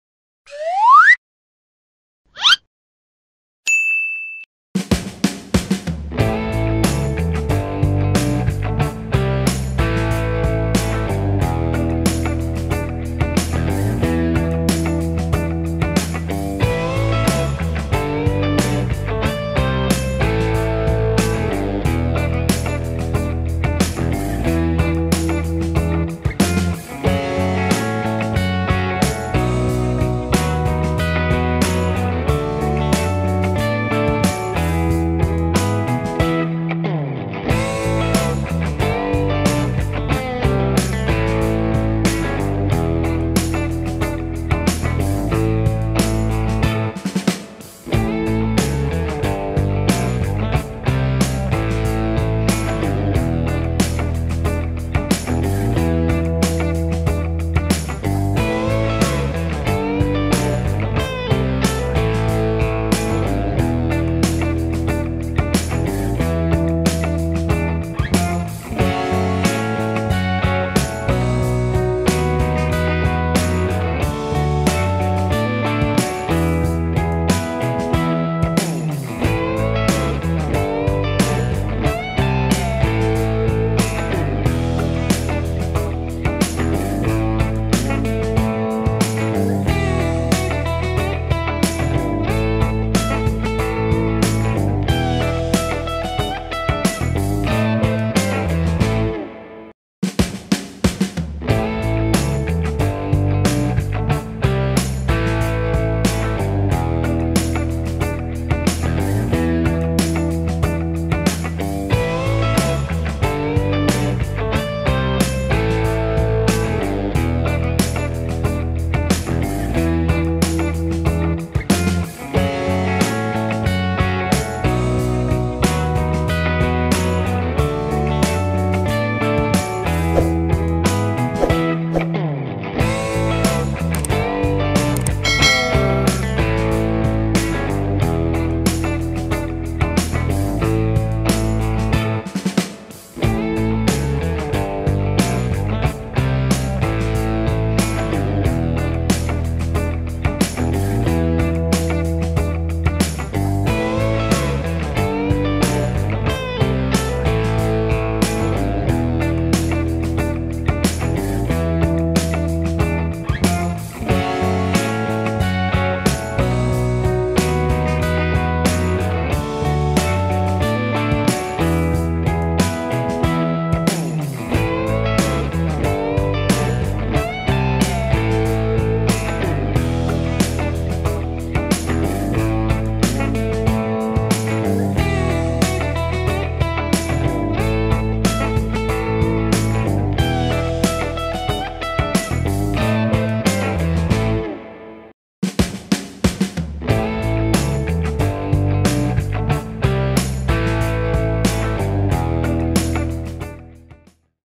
Drawing and Coloring a Suzuki Ignis Car
How to draw vehicles, draw a Suzuki Ignis car. Learn to draw, paint and color the Suzuki Ignis car using animation. Drawing and coloring animation.